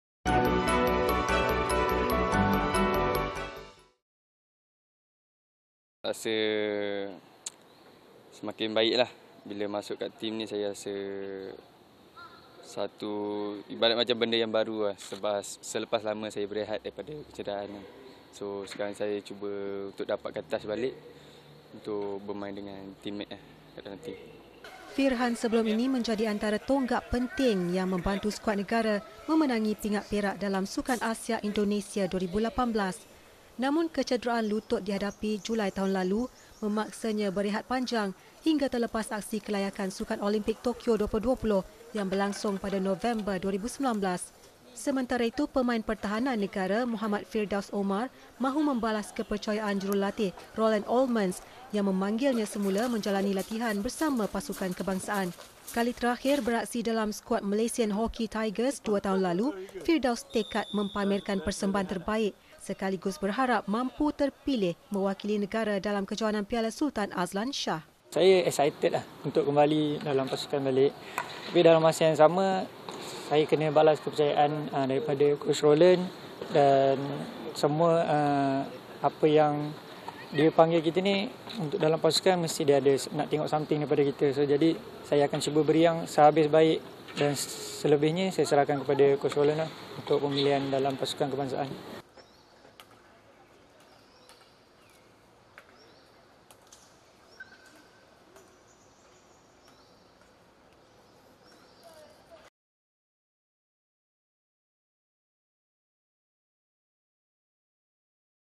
Firhan optimis pamer aksi terbaik
BuletinBernama : Selepas berehat 10 bulan ekoran kecederaan lutut kiri, penyerang utama skuad hoki negara, Firhan Ashari akui bergelut mendapatkan semula tahap kecergasannya.

Beliau bagaimanapun tidak gusar mengenai perkara itu, selain optimis mampu kembali ke prestasi terbaik menjelang Kejohanan Hoki Piala Sultan Azlan Shah September ini.